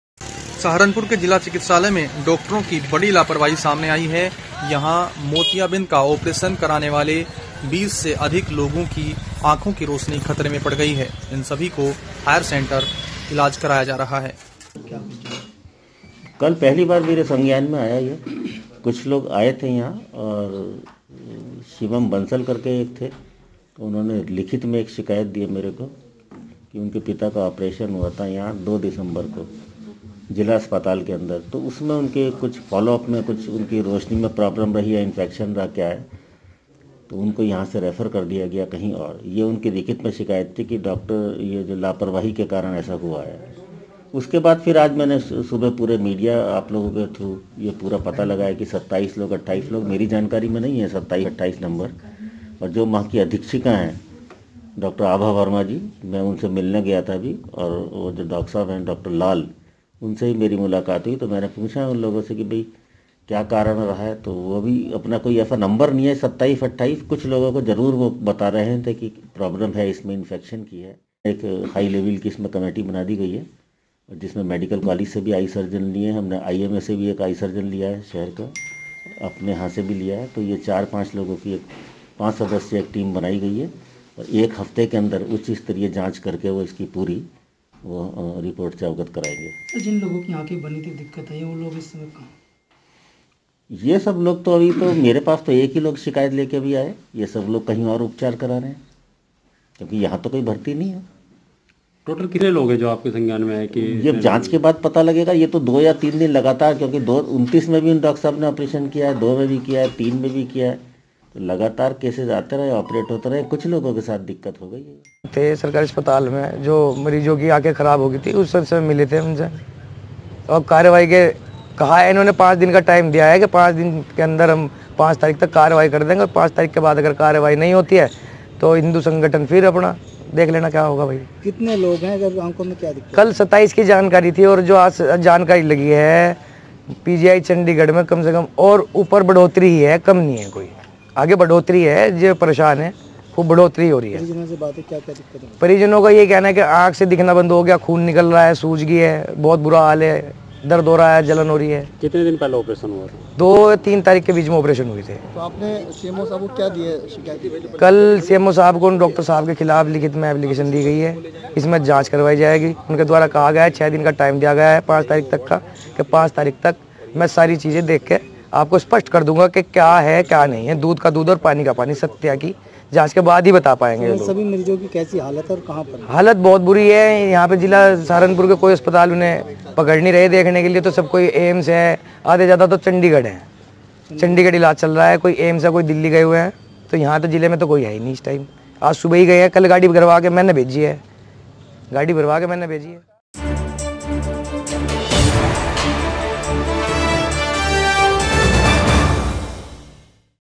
डॉक्टरों की बड़ी लापरवाही, मरीजों की आंखों की रोशनी खतरे में
उत्तर प्रदेश के सहारनपुर में डॉक्टरों की बड़ी लापरवाही सामने आई है। यहां जिला अस्पताल में मोतियाबिंद का ऑपरेशन कराने वाले कई मरीजों की आंखों की रोशनी खतरे में पड़ गई है। इनके परिजनों का आरोप है कि डॉक्टरों ने ऑपरेशन में लापरवाही की जिसके बाद मरीजों की आंखों में इंफेक्शन हो गया। इससे आंखों से खून आने लगा  और आंखों में सूजन आ गई। ऐसे पीड़ित रोगियों की संख्या 27 बताई जा रही है। मुख्य चिकित्सा अधिकारी ने पूरे प्रकरण की जांच के लिए सरकारी और गैर-सरकारी पांच डॉक्टरों की टीम गठित की है।